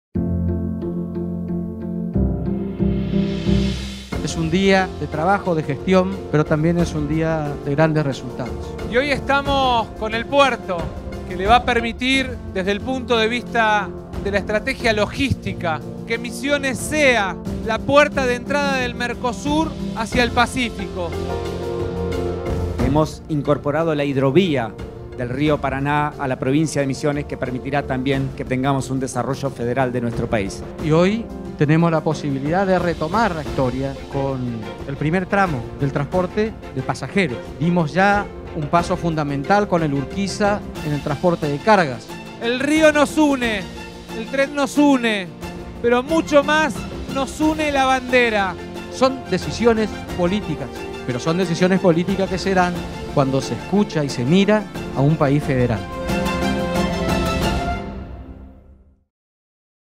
Massa y Meoni en Posadas. Más transporte para los misioneros
Video realizado en octubre de 2020.